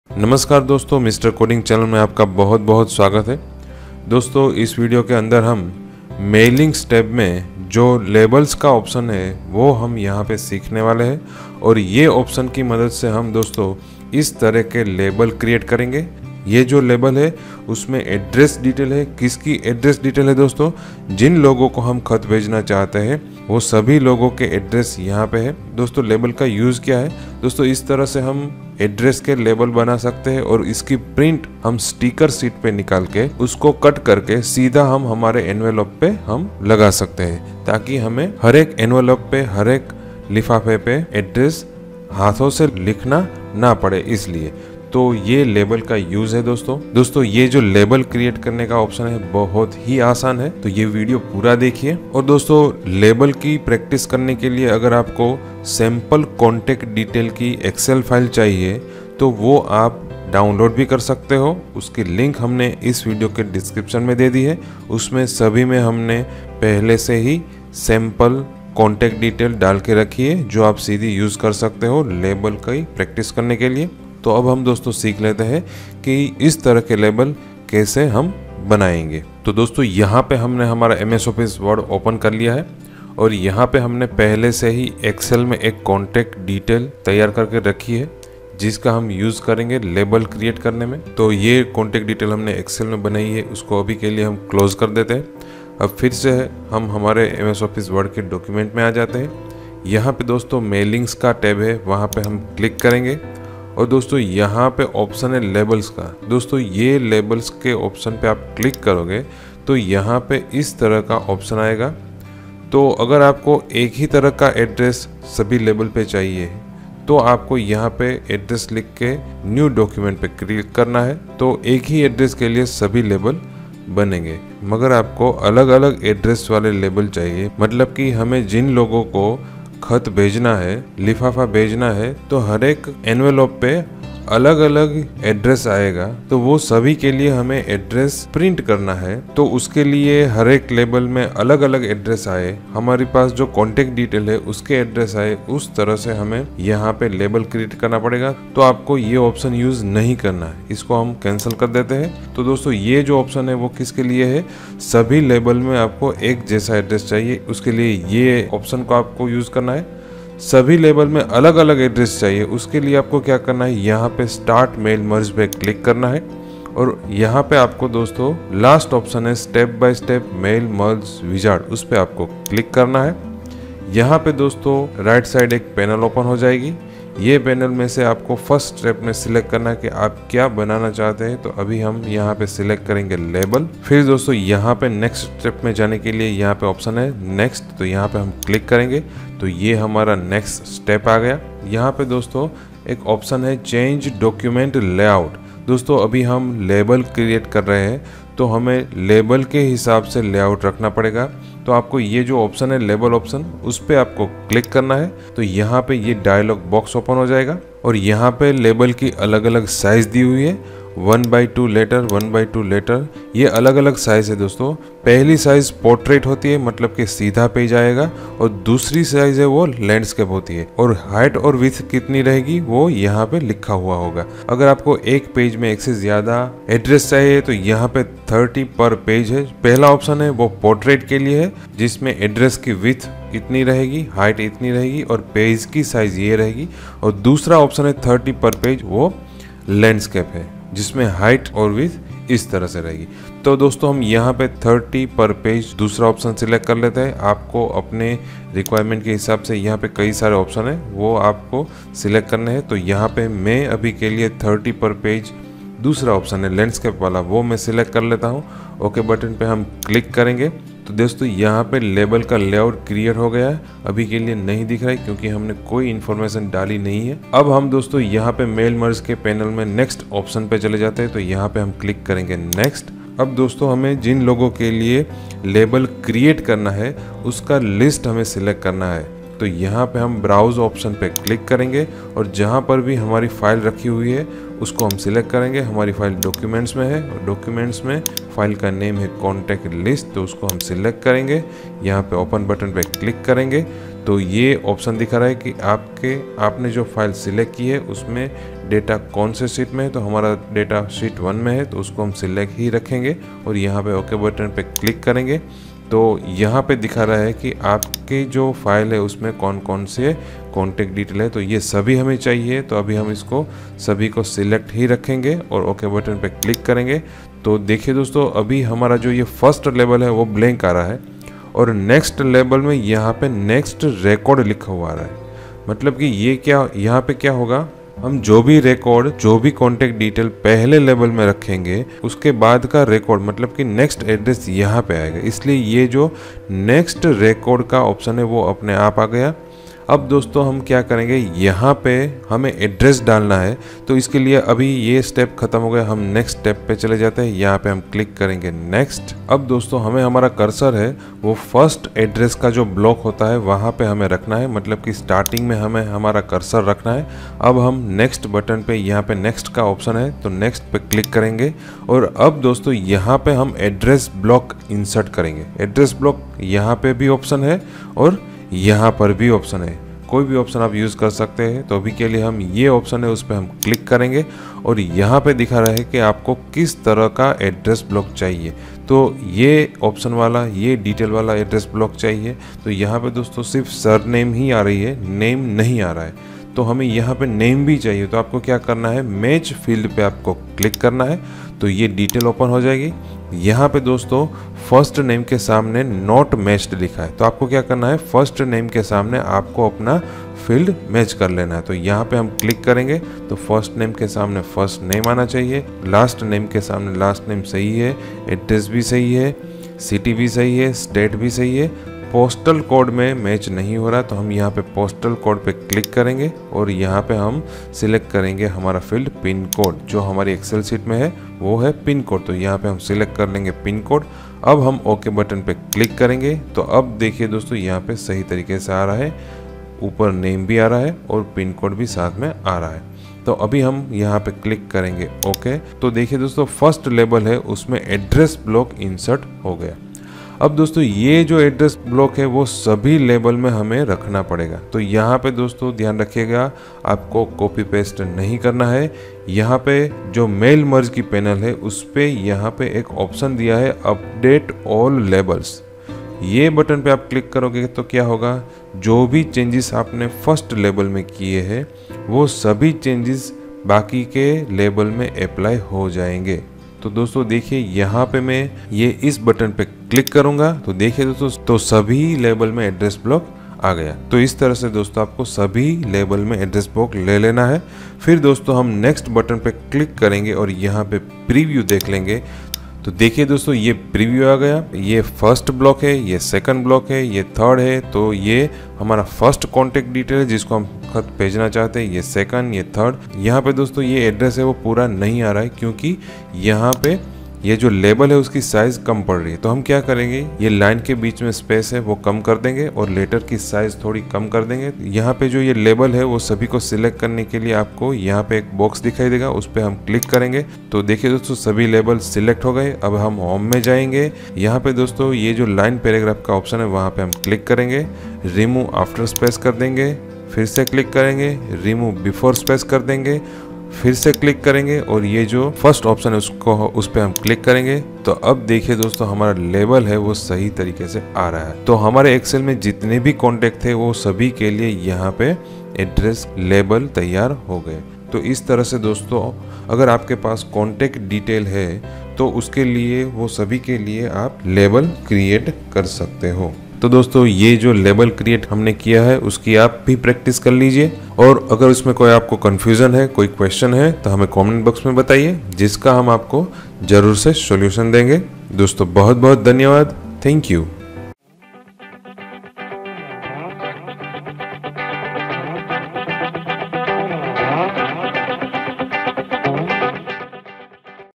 MSWORD-051-Labels | Create Address Labels Using Mail Merge | MS Word | Mailings Tab | Hindi Tutorial
MS Word Tutorial Part-051
लेबल | मेल मर्ज का उपयोग करके एड्रेस लेबल बनाएं  | मेलिंग टेब | हिंदी ट्यूटोरियल

Download MS Excel file for Sample Contact Detail
कॉन्टेक्ट डिटेल की सेम्पल एमएस एक्सेल फ़ाइल डाउनलोड करें

Hi,
Welcome to Mr. Coding Channel to learn something new.
In this video, we will learn below given topics
What are Labels?
What is the Use of Labels option in MS Word?
How to Create a single or 30 Address Labels per Page?
This is the only video tutorial in that you will find 100% practical examples.
and with the help of this video, you can easily able to create a Sticker sheet of Address labels in MS Word?
this option is available in the "Create" group of the Mailings tab in MS Word.
In this Hindi video tutorial, we create videos in such a way that anyone can easily understand the topic.
Then also if you have any queries then please comment with us, we will reply to you.

नमस्ते,
कुछ नया सीखने के लिए मिस्टर कोडिंग चैनल में आपका स्वागत है।
इस वीडियो में, हम नीचे दिए गए विषयों के बारे में जानेंगे
लेबल क्या हैं?
एमएस वर्ड में लेबल्स ऑप्शन क्या उपयोग है?
एक पेज में सिंगल या 30 एड्रेस लेबल कैसे बनाएं?
यह एकमात्र वीडियो ट्यूटोरियल है जिसमें आपको 100% व्यावहारिक उदाहरण मिलेंगे।
और इस वीडियो की मदद से आप आसानी से एमएस वर्ड में एड्रेस लेबल की स्टिकर शीट बना सकते हैं?
यह विकल्प एमएस वर्ड में मेलिंग टैब के "क्रिएट" ग्रुप में उपलब्ध है।
इस हिंदी वीडियो ट्यूटोरियल में, हम इस तरह से वीडियो बनाते हैं कि कोई भी विषय को आसानी से समझ सके।
फिर भी यदि आपका कोई प्रश्न है तो कृपया हमारे साथ टिप्पणी करें, हम आपको उत्तर देंगे।